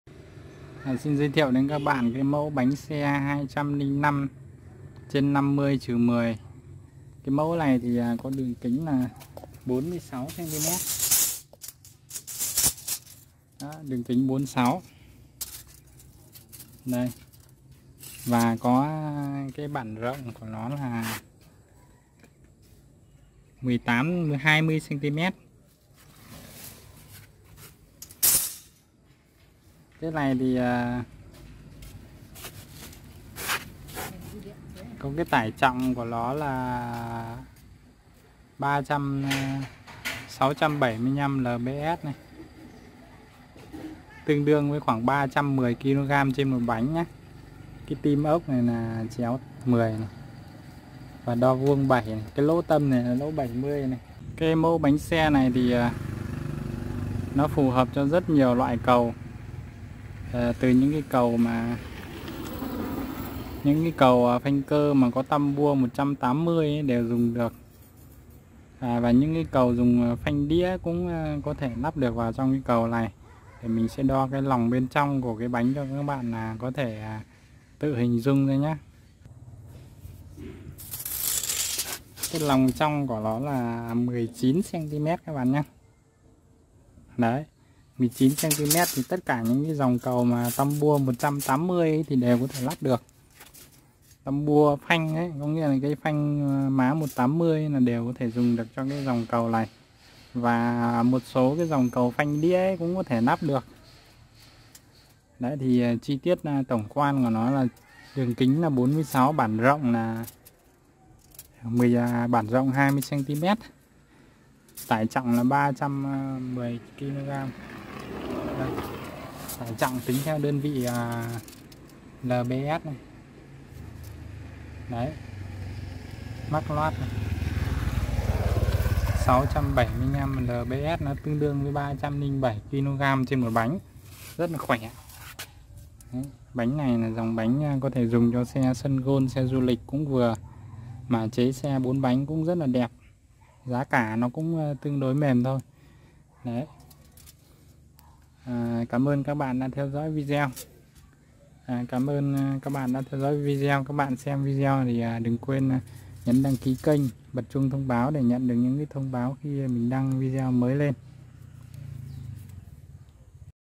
Mẫu bánh xe đẹp giá rẻ chế mọi loại xe 4 bánh đều đẹp tải trọng cao 205/50-10
Mẫu bánh xe đẹp giá rẻ chế mọi loại xe đều đẹp tải trọng cao
- Model: 205/50-10
- Đường kính 46cm, bản rộng 20cm
- Tải trọng 675Lbs ~ 307kg / 1 bánh
- Tim 4 ốc 7x10cm, lỗ tâm bánh 70mm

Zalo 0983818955